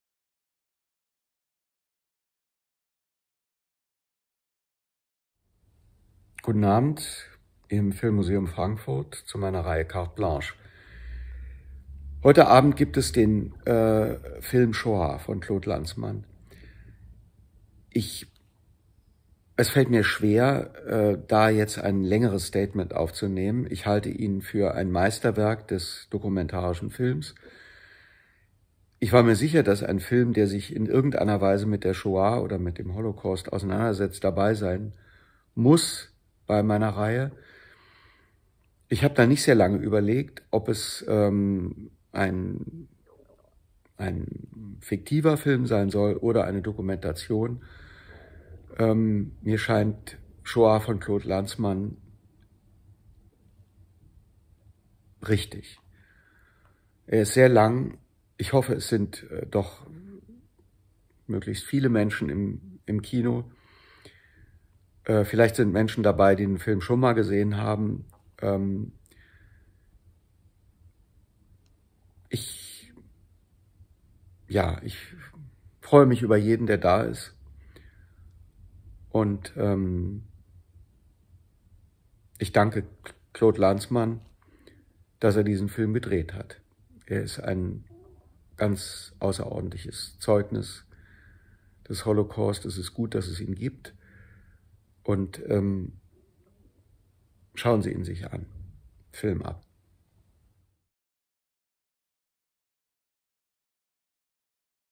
CARTE BLANCHE: ULRICH MATTHES // SHOAH (1985)
Die Carte Blanche im März 2022 ist Ulrich Matthes gewidmet: Einen Monat lang präsentiert der Schauspieler und Präsident der Deutschen Filmakademie die acht Filme, die ihn am meisten geprägt und beeinflusst haben. Anhand von Videobotschaften vor den Filmen erläutert er seine Auswahl.

SHOAH
Frankreich/Großbritannien 1985. R: Claude Lanzmann. Dokumentarfilm. 35mm. OmU
Original version with German subtitles

Mehr als zehn Jahre arbeitete Claude Lanzmann an SHOAH, einem der umfassendsten Filme über die Ermordung der Juden im Nationalsozialismus. Lanzmann verwendet weder Archivmaterial noch Kommentare. Er kehrt an die Orte der Vernichtung zurück und spricht mit Opfern und Täter:innen über die „Radikalität des Todes“. Die insistierende Frage nach dem Wie statt dem Warum entlockt den Interviewpartnern Dinge, über die sie nie zuvor gesprochen haben.

„Ein Meisterwerk des dokumentarischen Films. Bei meiner Reihe musste ein Film dabei sein, der sich mit dem Holocaust auseinandersetzt.“ (Ulrich Matthes)